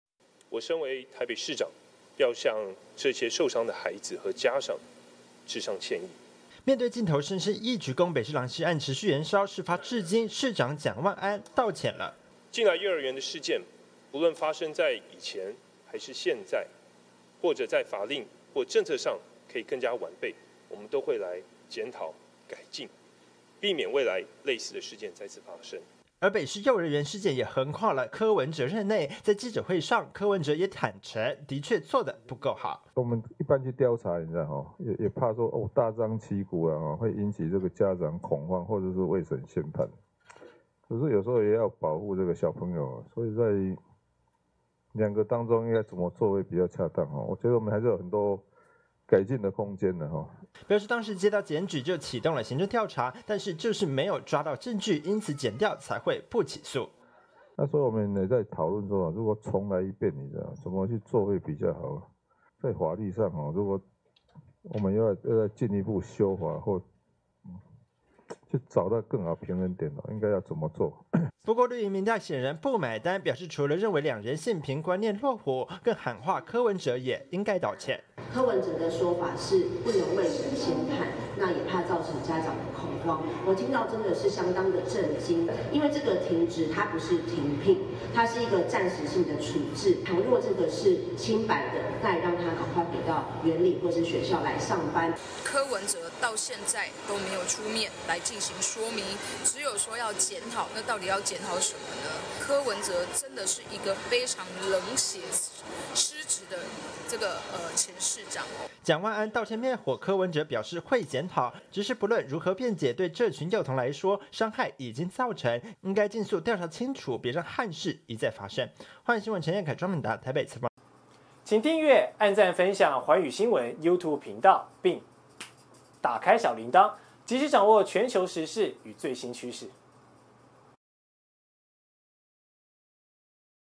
北市幼兒園狼師案延燒！蔣萬安「鞠躬道歉」：會檢討｜寰宇新聞
北市幼兒園狼師案持續延燒，台北市長蔣萬安，上午向國人鞠躬道歉，表示身為北市的大家長，責任就是保護孩子不受傷害，會就政策、法令全面檢討，前台北市長柯文哲，對此在記者會上也承認有改進空間，應該進一步修法。不過，民進黨立委不買單，怒轟兩任市長性平觀念落伍，柯文哲是一個冷血且失職的市長。